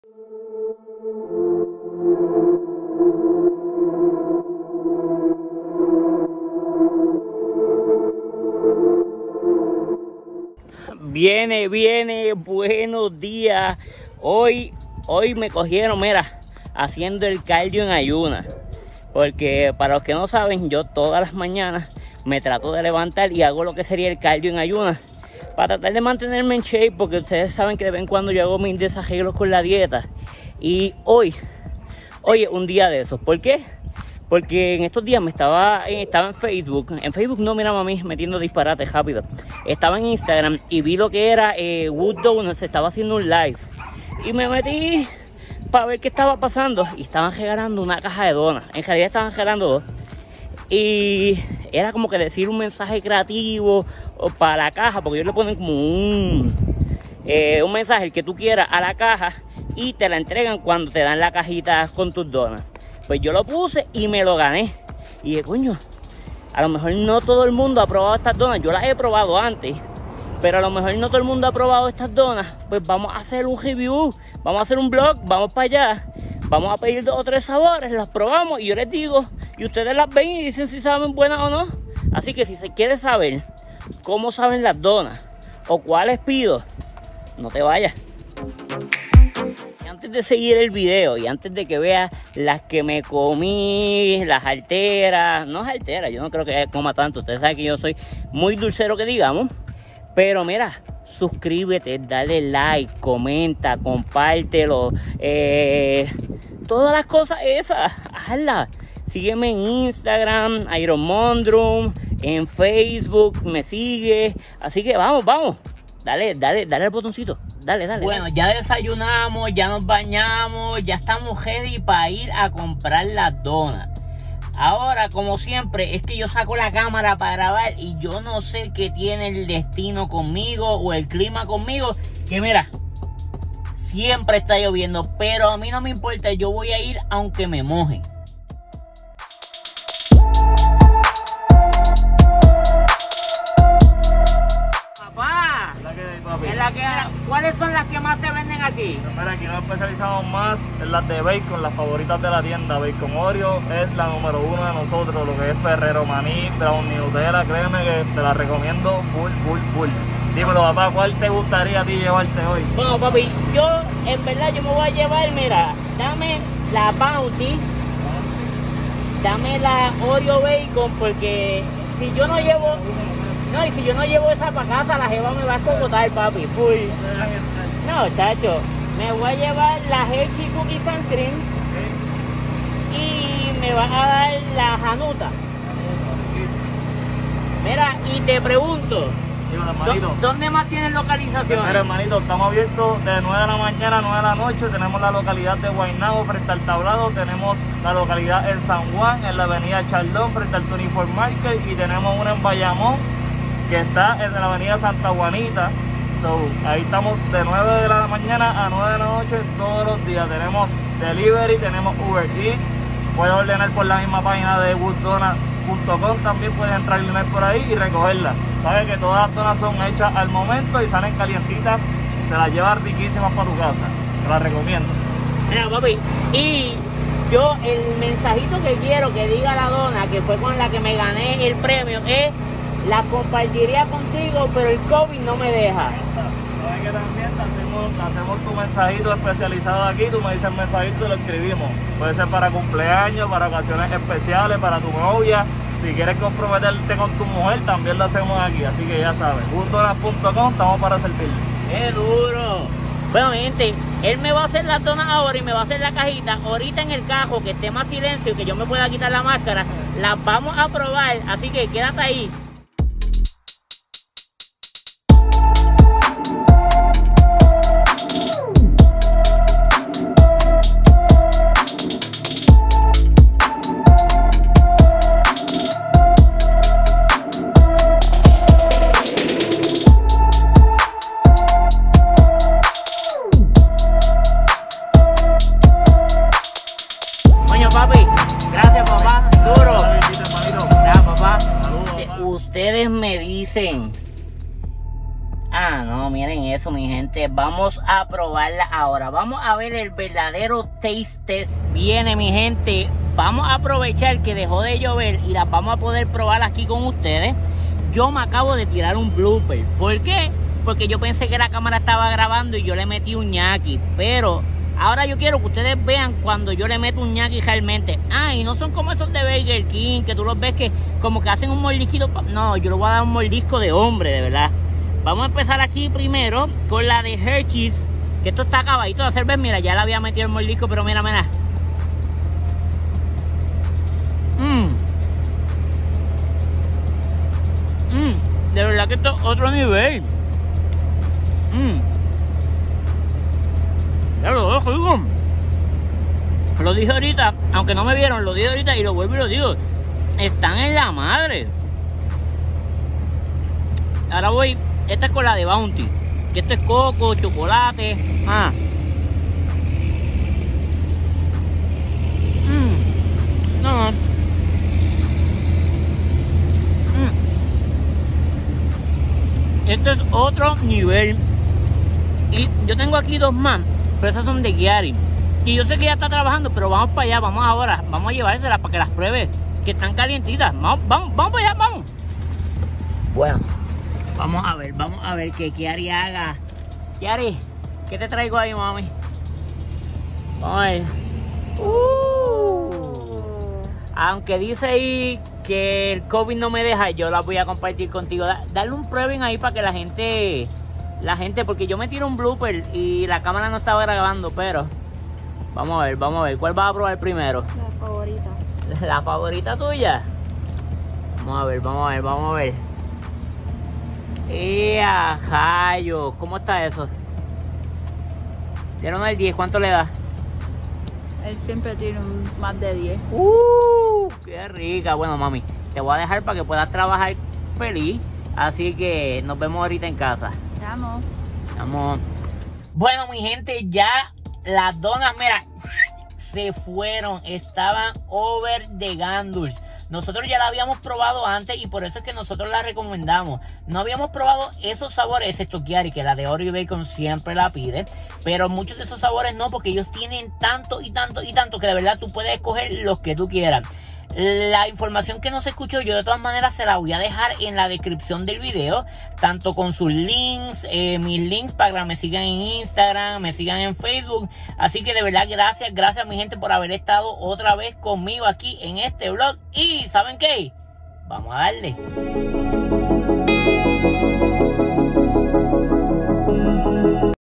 Vlog | Probando las Donas de Wooddoughnuts | me quede con ganas de comermelas todas 😭
Bueno mi gente me di la tarea de probar algunas de las donas de wooddoughnuts y tengo que decirles que están a otro nivel, y miren que yo no soy dulcero, pero saben tan buenas, no son empalagosas ni grasosas son al horno. Se las recomiendo a ojos cerrados. Les voy a dejar aquí sus redes para que ven todos sus sabores y sus localidades.

Ofertas: 4 x $7.50
6 x $10
12 x $20